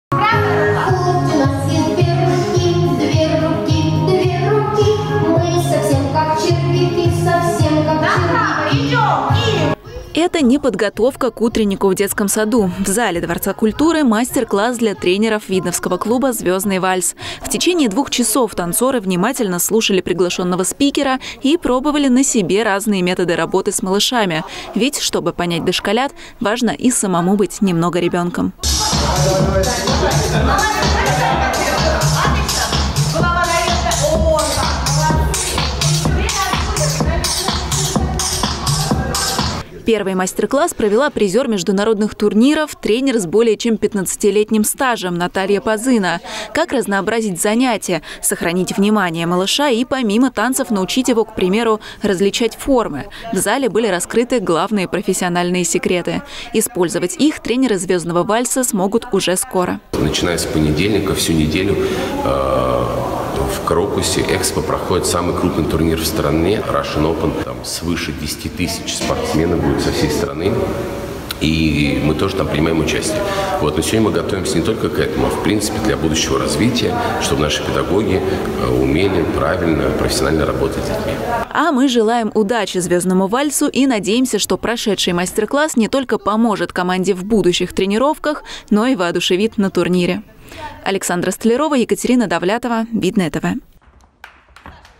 ОБМЕН ОПЫТОМ
Регулярный обмен опытом между тренерами – залог успеха видновского клуба «Звездный вальс». Во Дворце культуры в течение пяти часов проходили мастер-классы по работе с детьми разных возрастов.